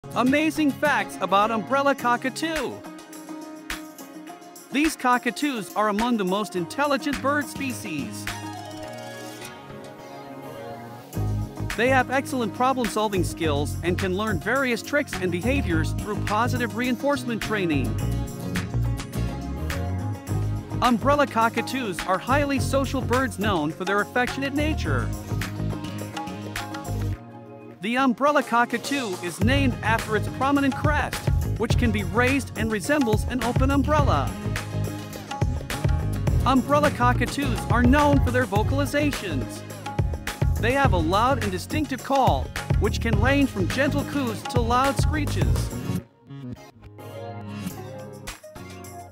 Amazing Umbrella Cockatoo Facts
The umbrella cockatoo (Cacatua alba) is a captivating and intelligent bird that can make a wonderful companion for the right owner. With their striking appearance, charming personality, and unique vocalizations, they have gained popularity among bird enthusiasts. However, owning an umbrella cockatoo requires a significant commitment of time, effort, and resources.